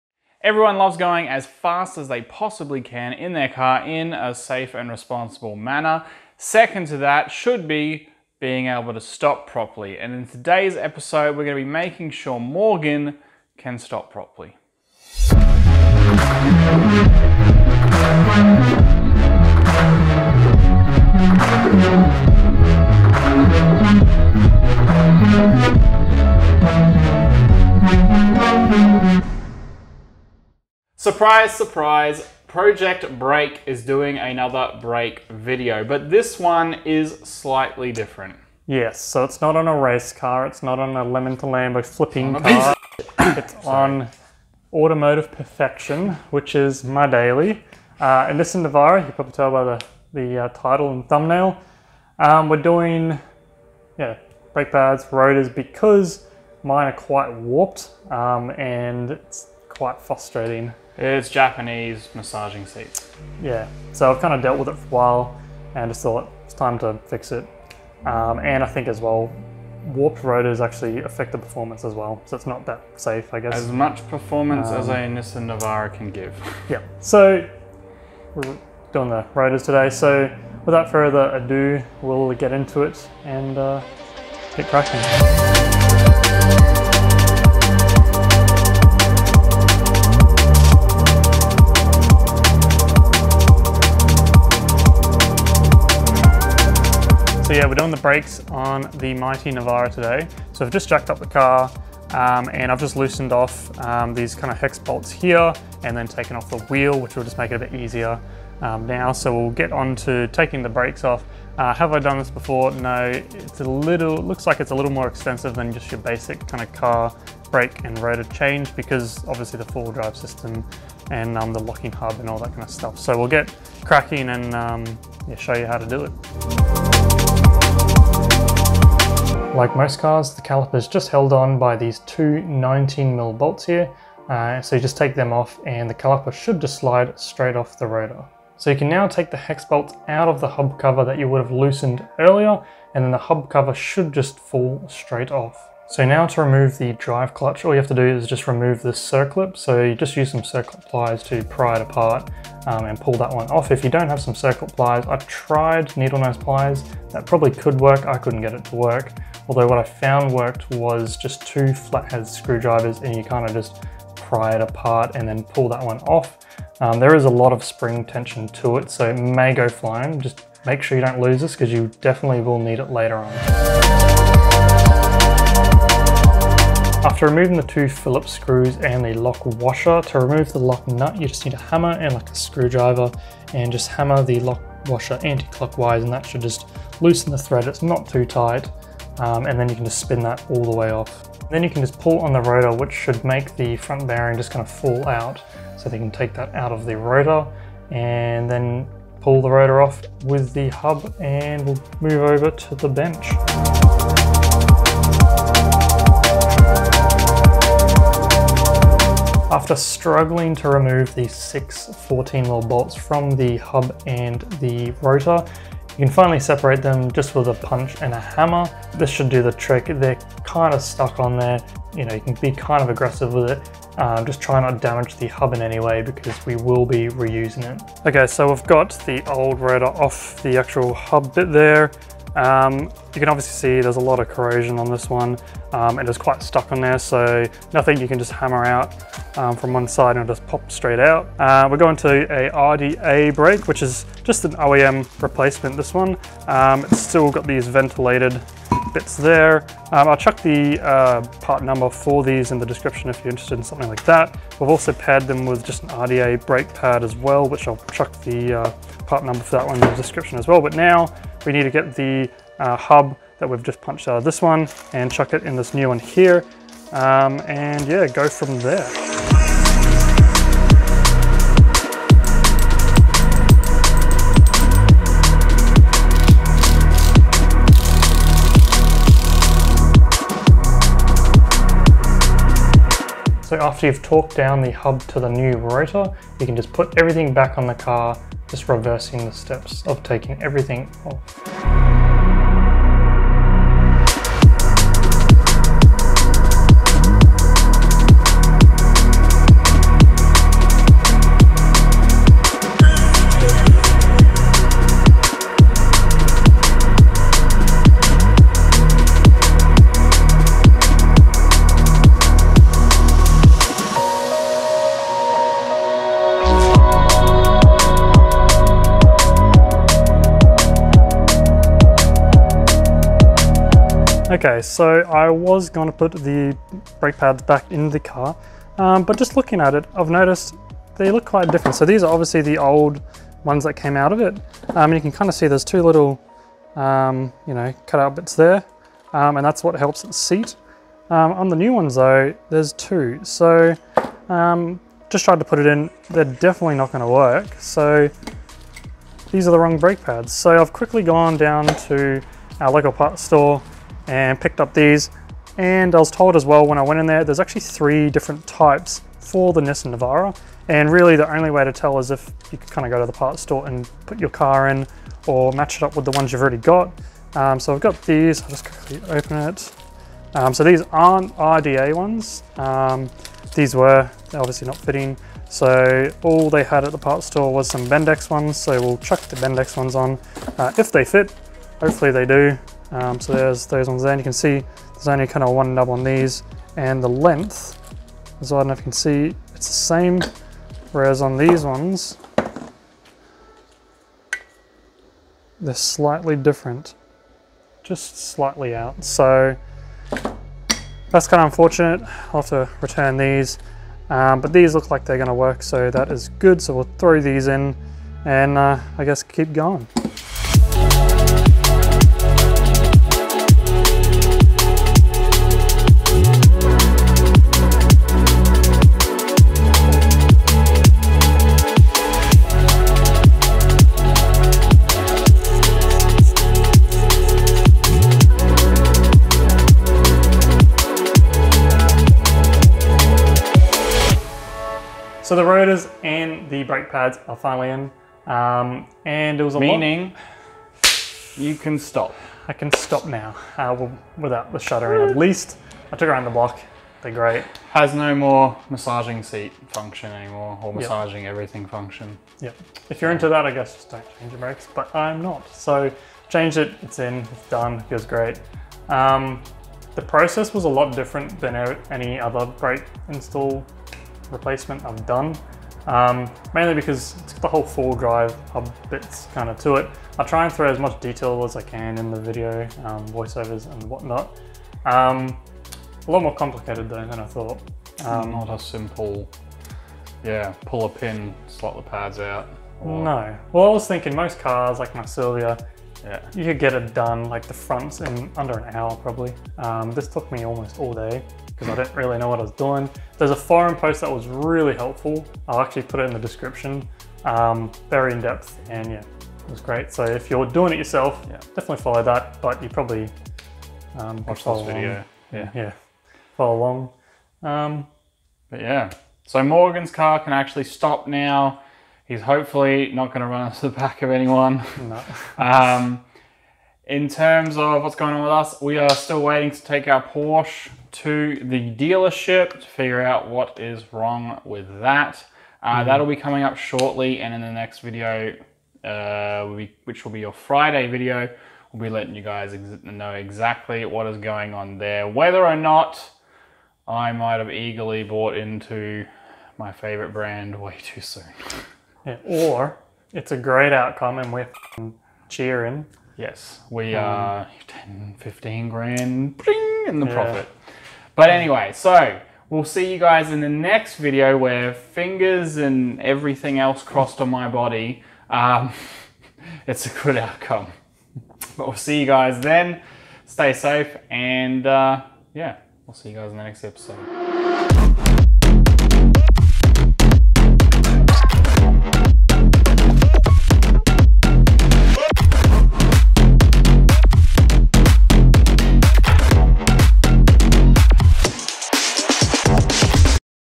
How to Replace Brake Pads and Rotors on a Nissan Navara D22 4X4 (Frontier)
In this video, Morgan will show you how to replace your brake pads and rotors on your Nissan Navara D22 4x4 (Frontier). It's a fairly simple process that can save you a lot of money on mechanic's fees, and it's something that anyone with a basic set of tools can do at home.

Parts Used:
- RDA7655 (Rotors)
- RDX2438SM (Pads)
- Automotive grease to regrease internal moving parts in the hub (Suggested)

Whiteline discount:
Get $20 off your first purchase when you spend $150 or more by using our code WL4317.

Artwork By:
Andrew Babin

Chapters:
00:00 - Intro
00:31 - Explanation
01:24 - Jack Up Your Car & Remove Wheel
01:38 - Plan of Attack
02:06 - Remove Brake Calliper from Spindle
02:18 - Removing Hub Cover & Drive Clutch
02:54 - Removing Front Bearing
03:23 - Separating Rotor From Hub
03:48 - Rotor Comparison
04:29 - Installing Hub Into New Rotor
04:51 - Installing Rotor, Front Bearing & Hub
05:25 - Pad Differences (3 Different Styles)
06:50 - Installing Pads
07:18 - Summary
09:27 - Outro